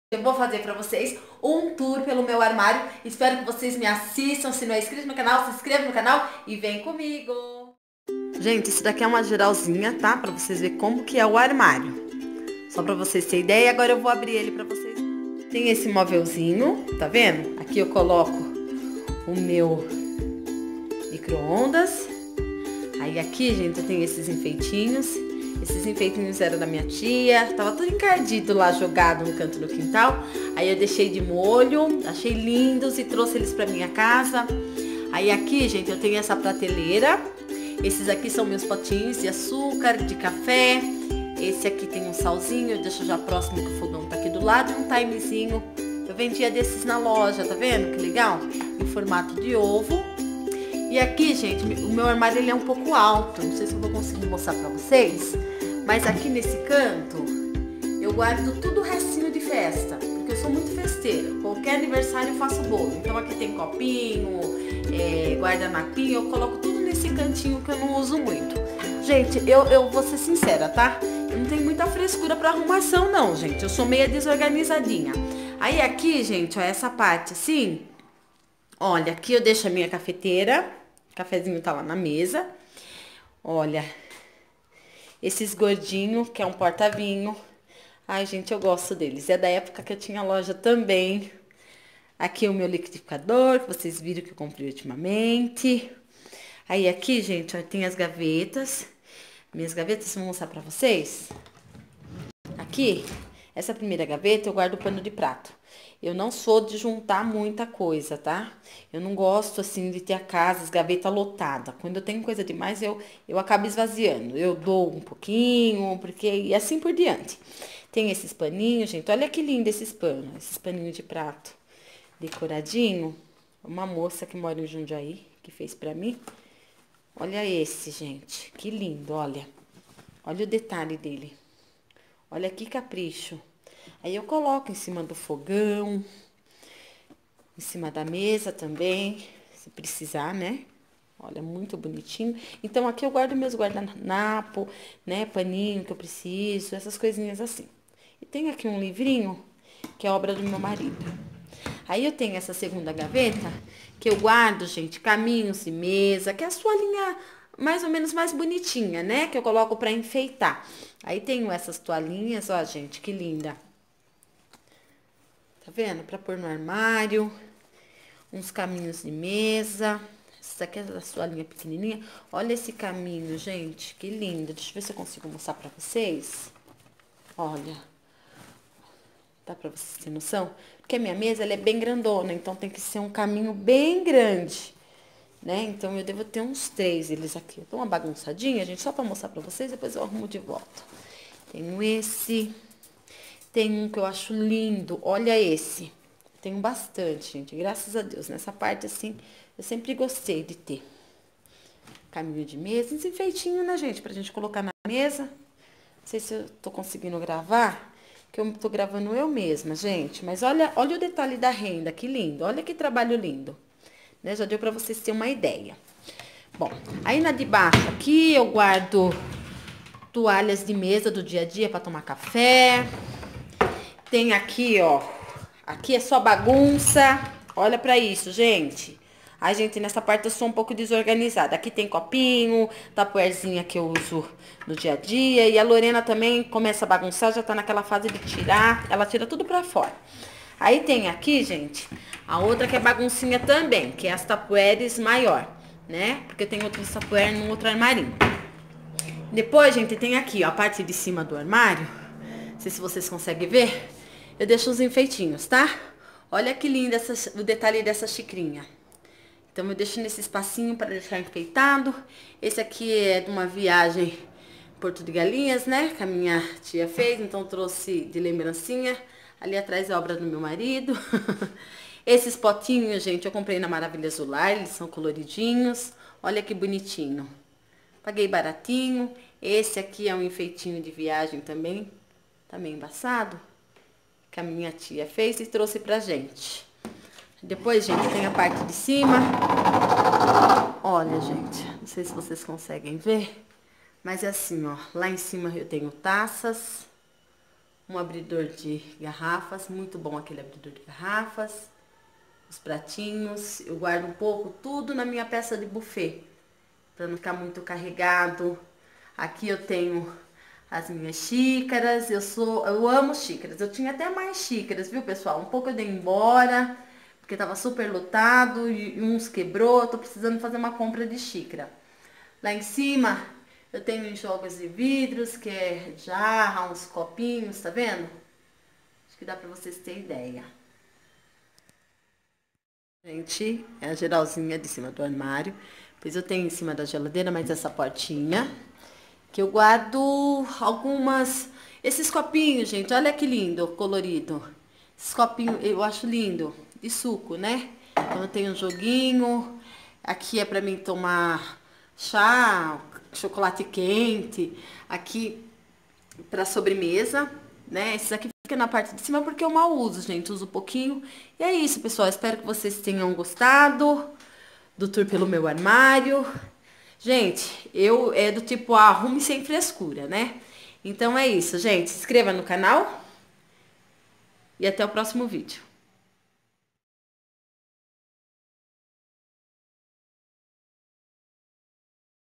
TOUR pelo Armário da cozinha
tourdoarmário abrindoarmário Familia abençoada,vou fazer um tour do armário da cozinha,mostrar como mantenho toda a organização.